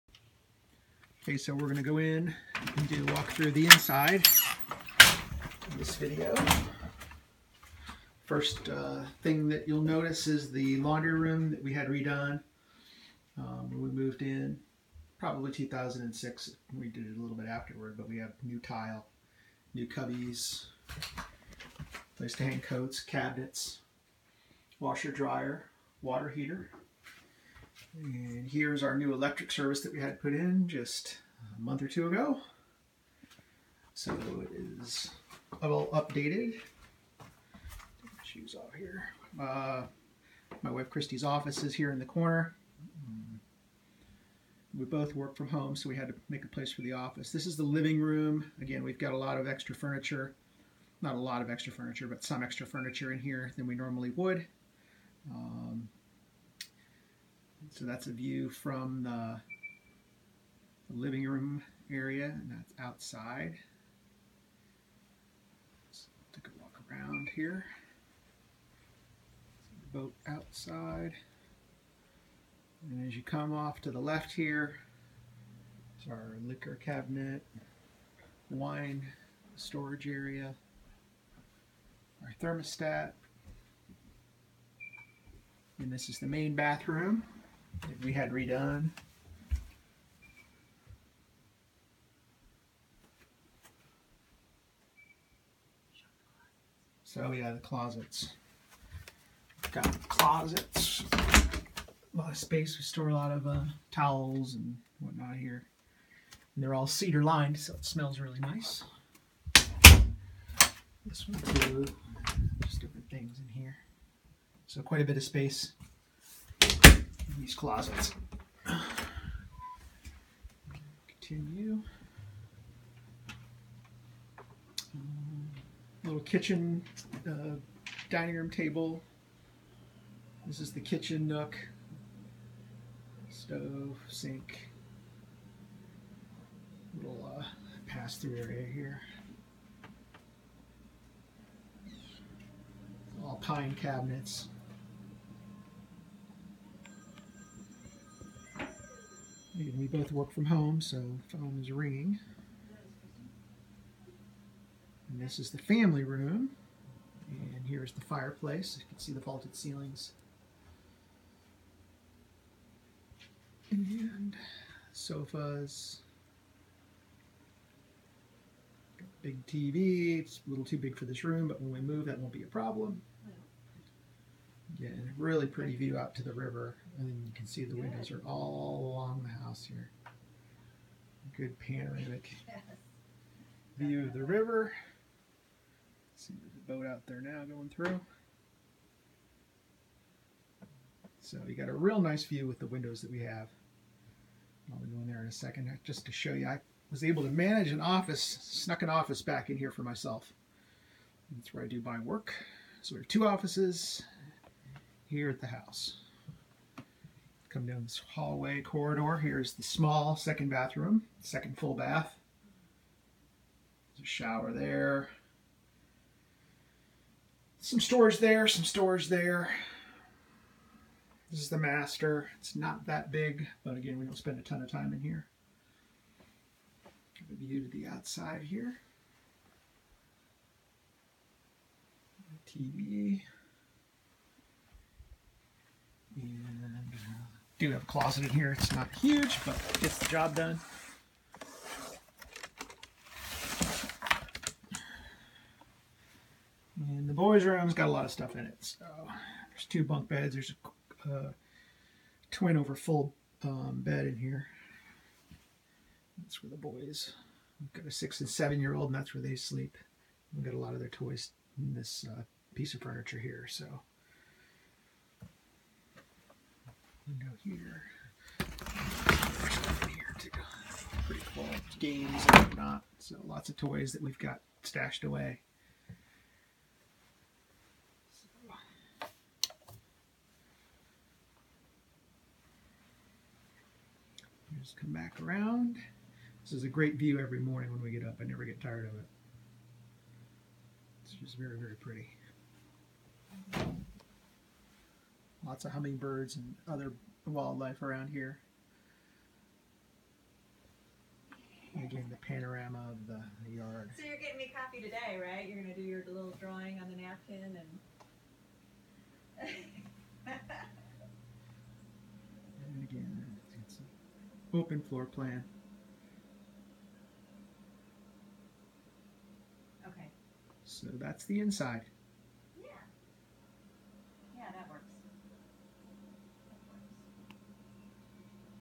8178 Devils Elbow
A short tour of the inside of the house.